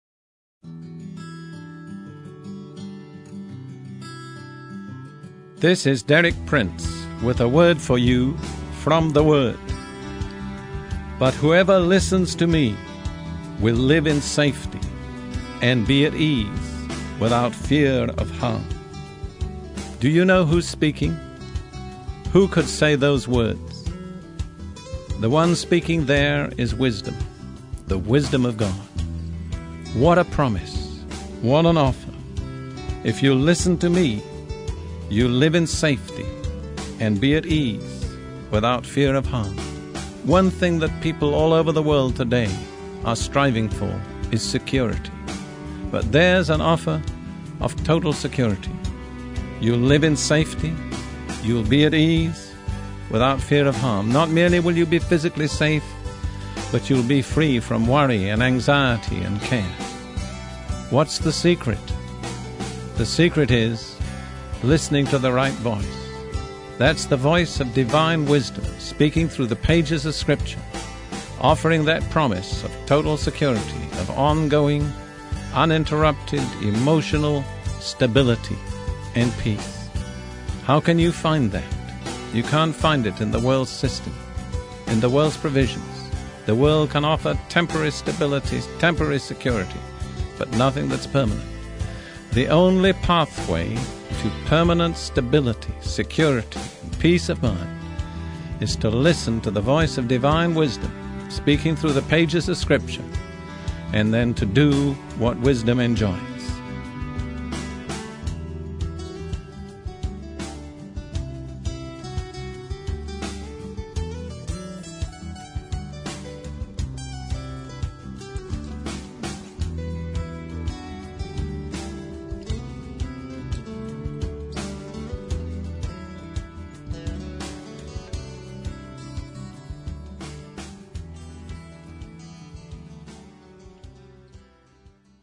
Live In Safety | Part 145 | Derek Prince Devotions
In these devotions, Derek Prince shares an encouraging word from the Word of God. While you’re listening, enjoy the beautiful aerial footage that’s been filmed in the land of Israel. May it be a blessing to you!